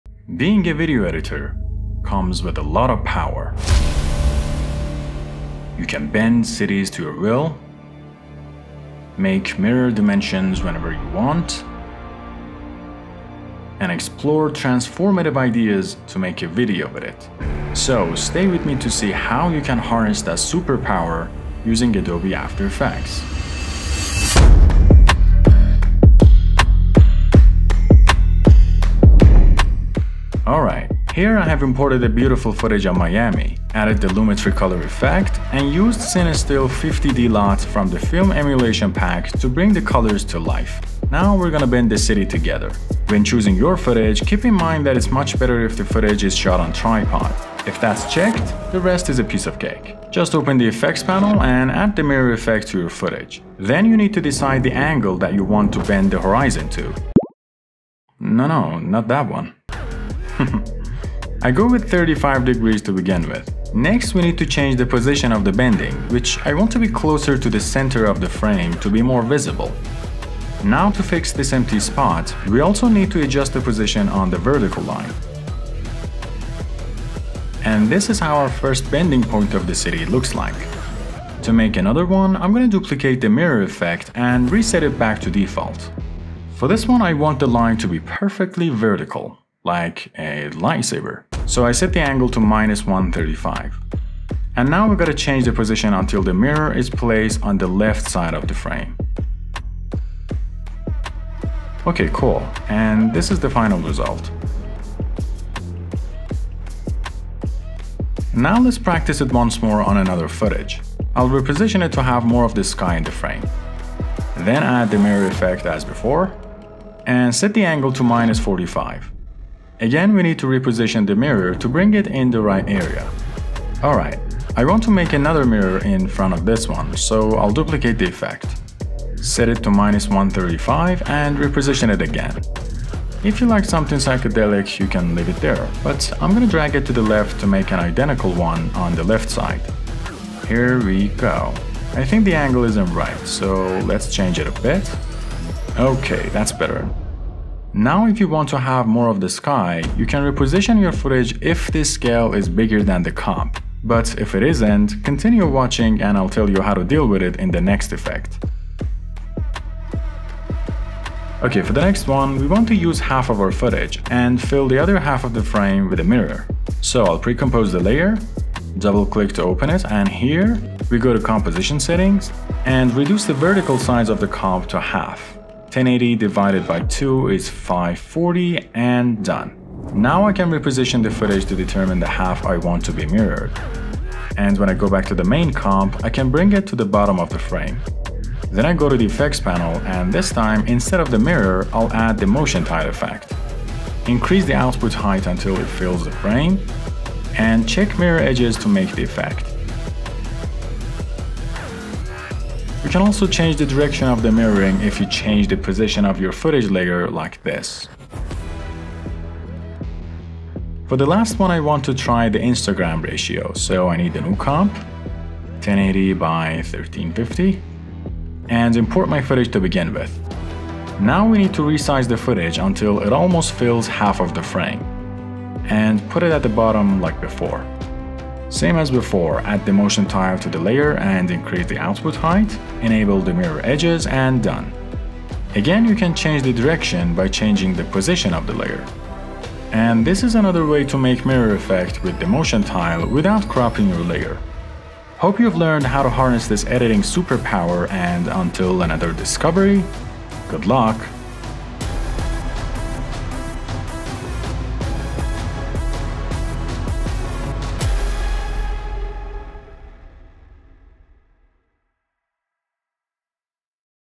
Bend The Scene Like Inception! | After Effects Tutorial
In this video, we'll be showing you three professional tricks for creating the mirror effect in Adobe After Effects. Whether you're a beginner or an experienced user, these tips will help you take your mirror effect shot to the next level. We'll show you how to create a realistic and dynamic mirror effect in After Effects. So sit back, relax, and let's get started!

🎅🎁 Holiday Mega Sale
This is your one shot to stack your 2026 projects with every template, effect, and tool we’ve got.